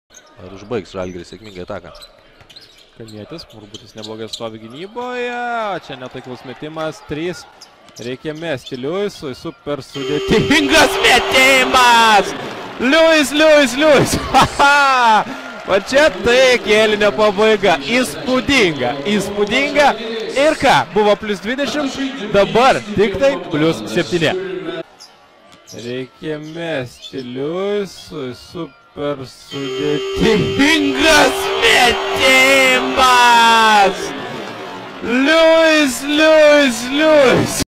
Ch.Lewis - very difficult shot from halfcourt!!
Juventus Utena player Chavaughn Lewis - very difficult shot from halfcourt!! (vs Zalgiris, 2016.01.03)